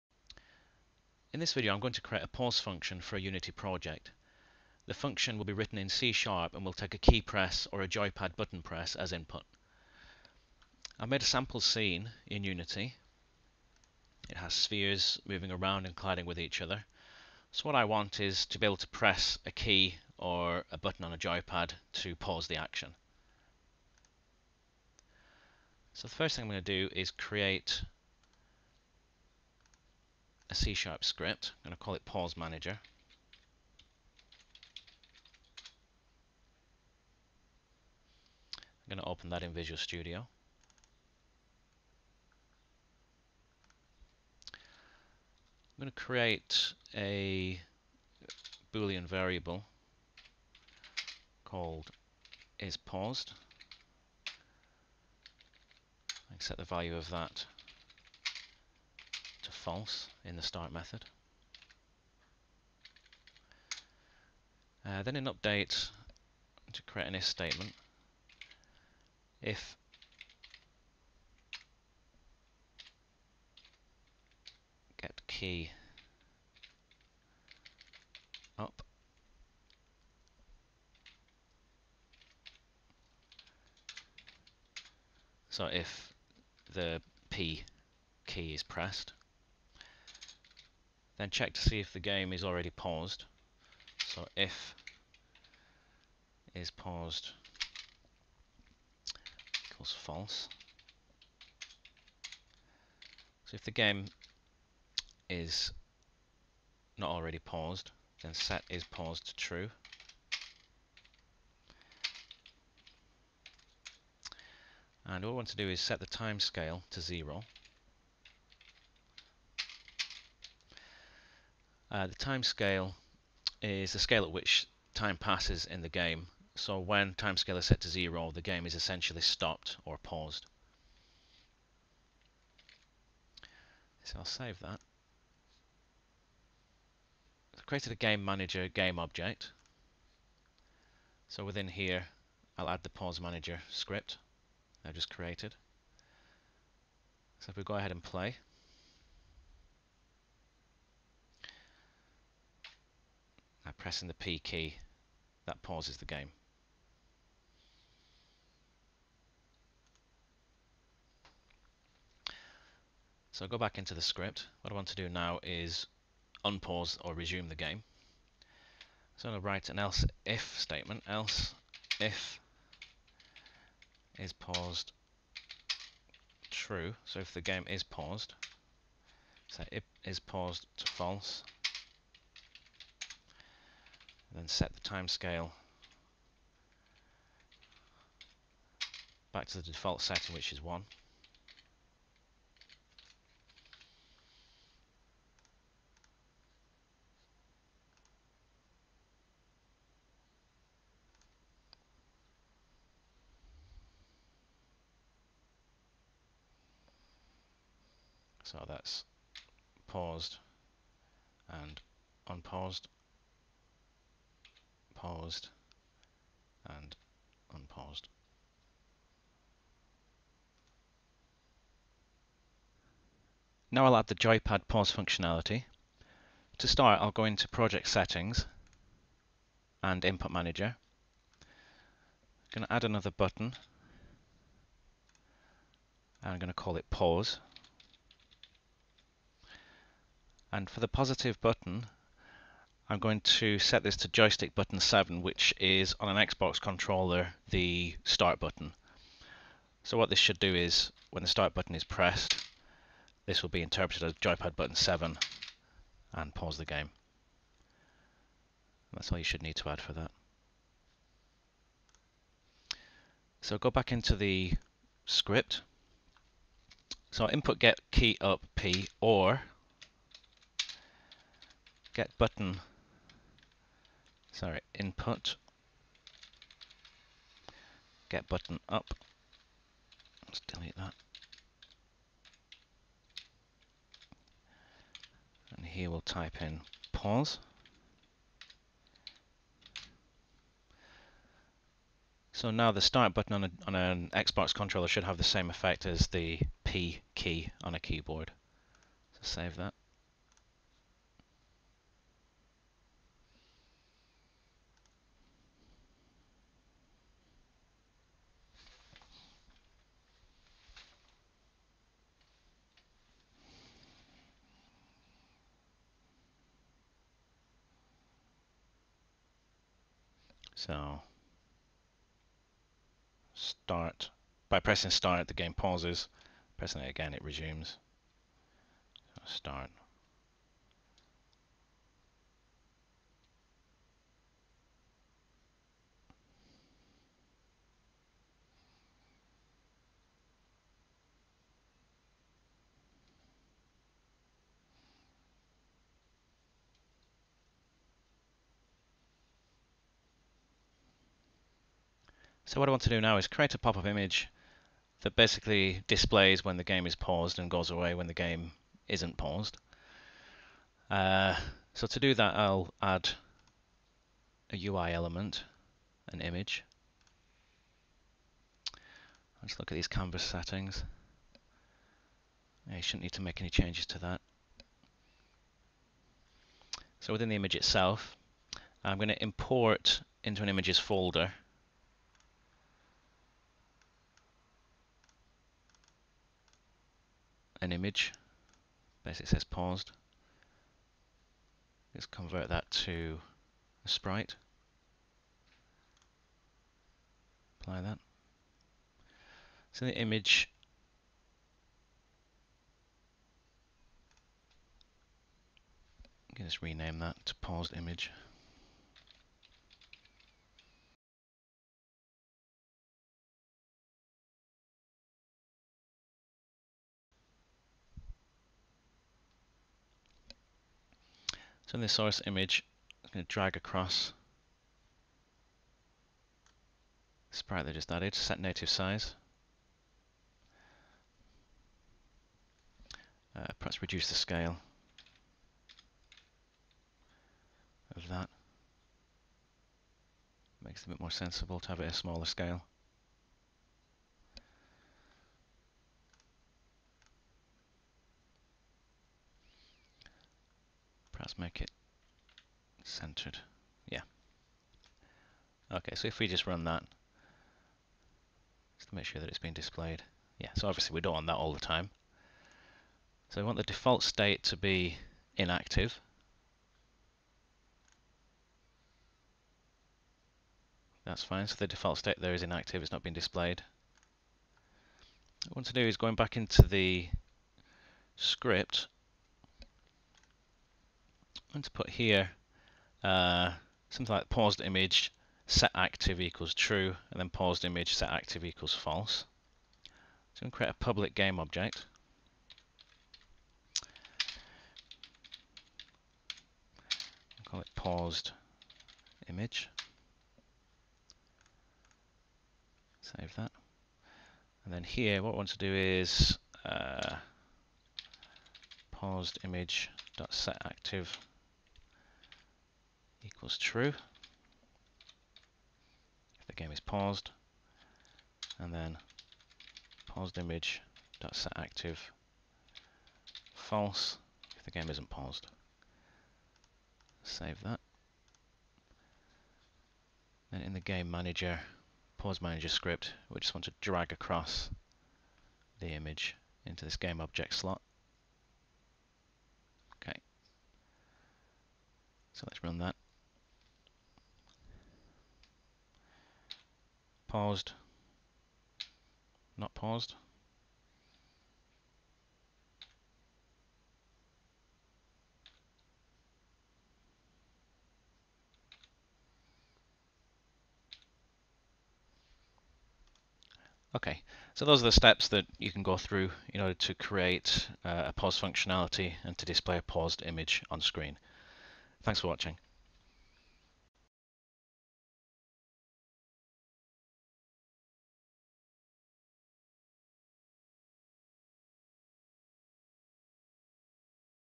Coding a game PAUSED Function with Unity and C#
In this video I use C# to code a pause function for a game developed in Unity. The function works with keyboard and joypad, and displays a "PAUSED" image when the game is paused.